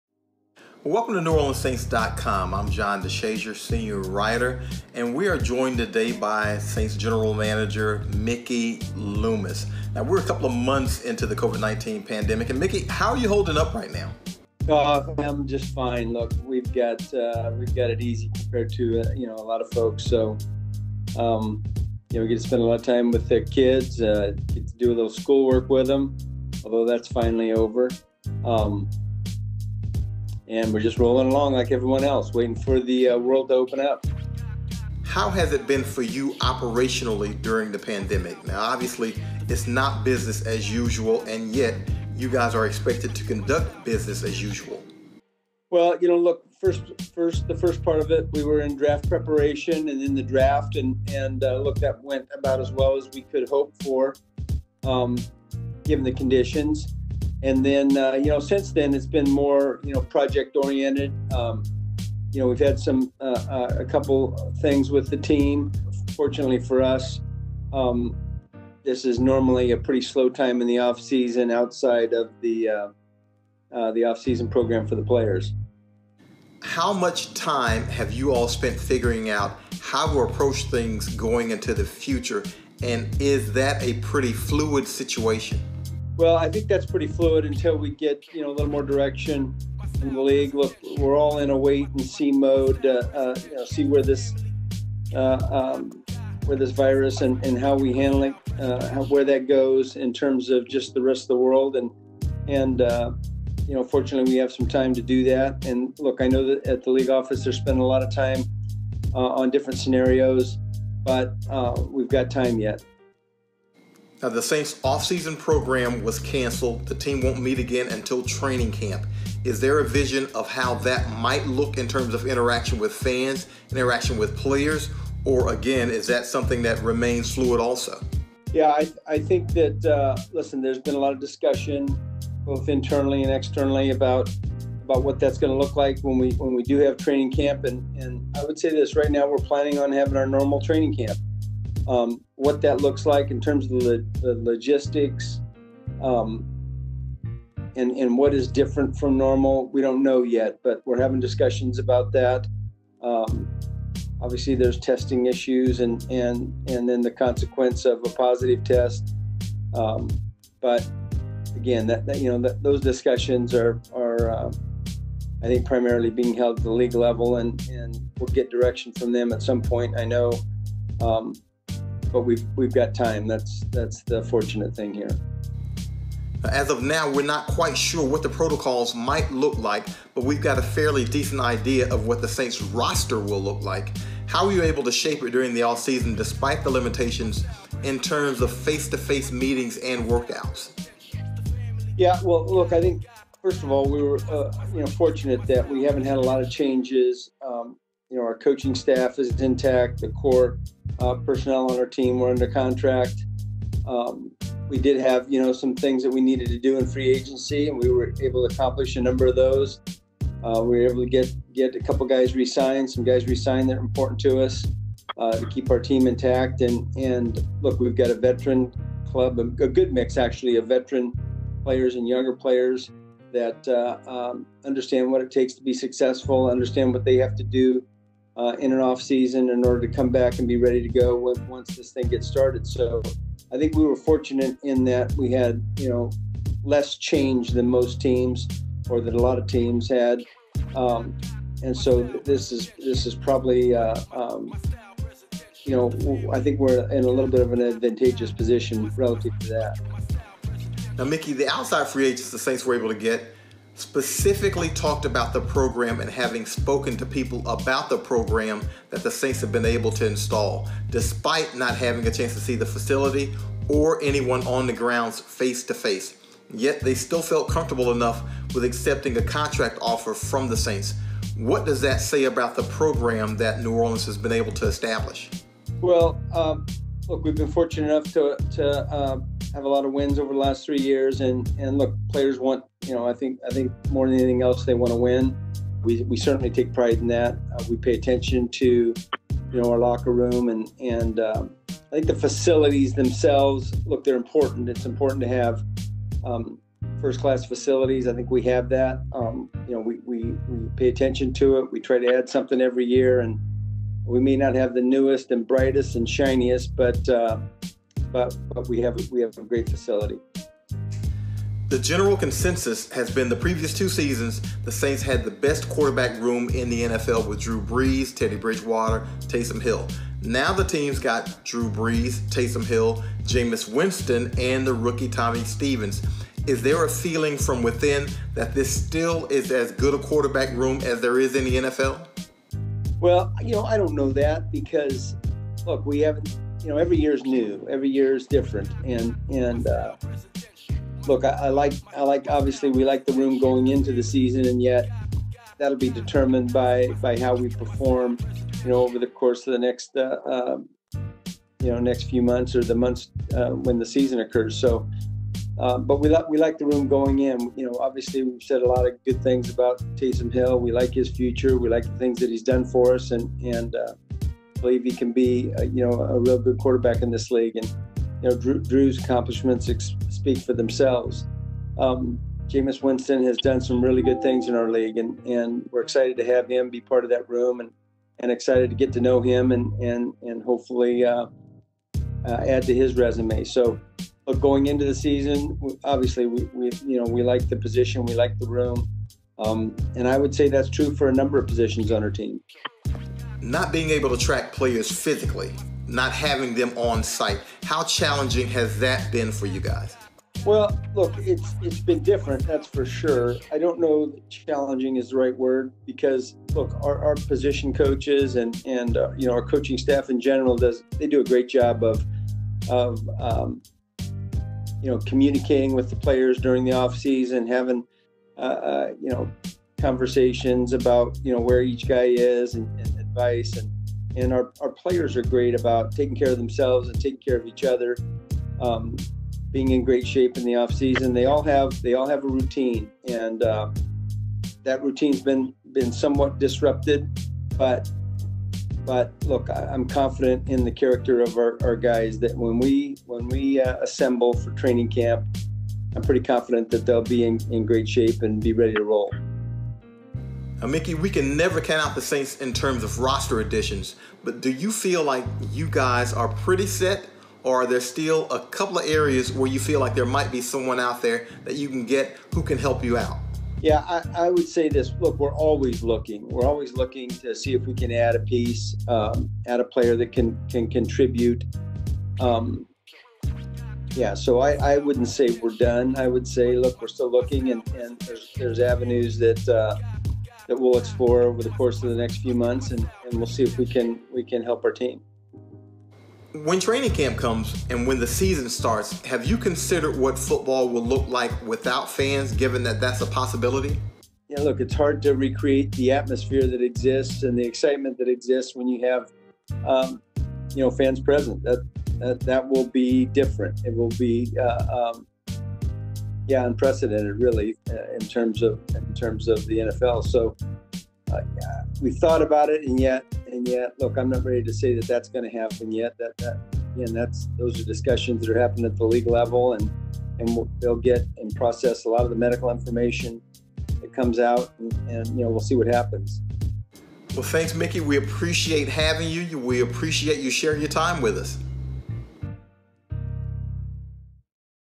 GM Mickey Loomis: Saints planning on normal training camp
New Orleans Saints GM Mickey Loomis talks about the unusual offseason and planning for training camp in an interview with John DeShazier of NewOrleansSaints.com.